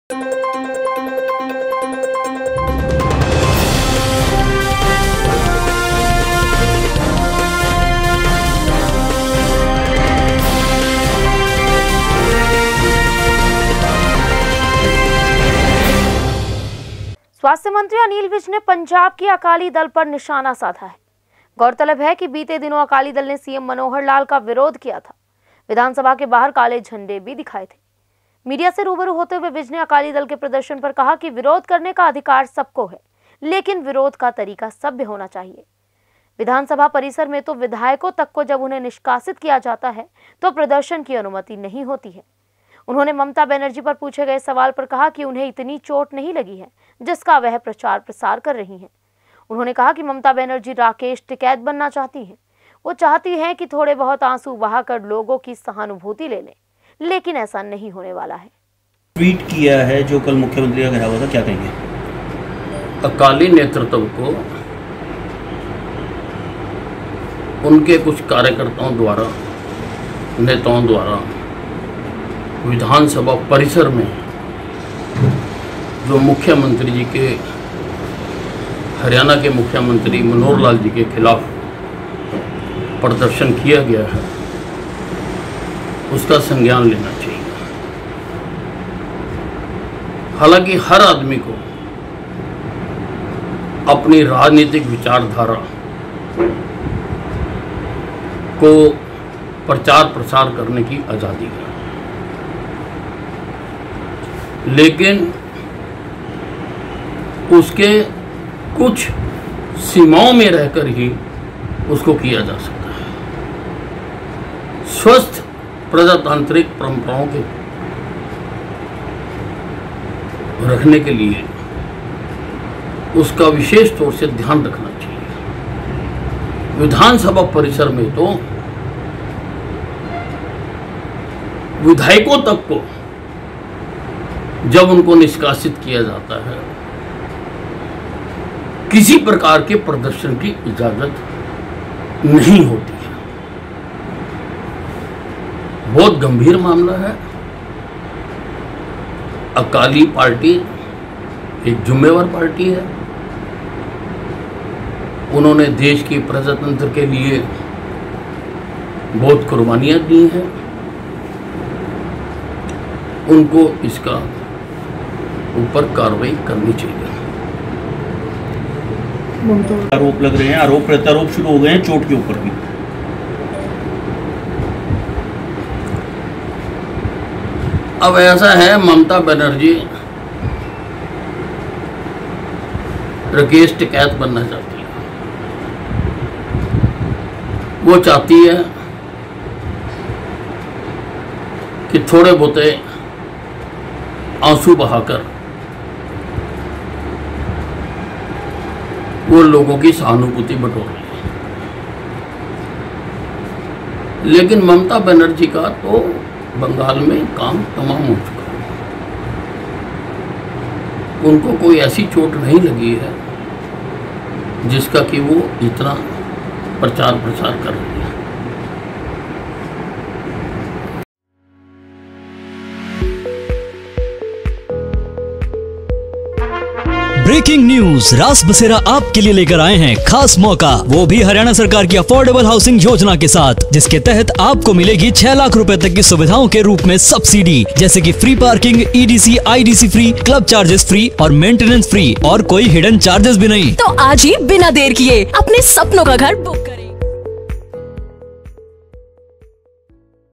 ममता बनर्जी बनना चाहती हैं राकेश टिकैत
स्वास्थय मंत्री अनिल विज ने पंजाब की अकाली दल पर निशाना साधा है। गौरतलब है कि बीते दिनों अकाली दल ने सीएम मनोहर लाल का विरोध किया था। विधानसभा के बाहर काले झंडे भी दिखाए थे। मीडिया से रूबरू होते हुए विज ने अकाली दल के प्रदर्शन पर कहा कि विरोध करने का सबको अधिकार है, लेकिन विरोध का तरीका सभ्य होना चाहिए। विधानसभा परिसर में तो विधायकों तक को जब उन्हें निष्कासित किया जाता है, उन्हें तक प्रदर्शन की अनुमति नहीं होती है।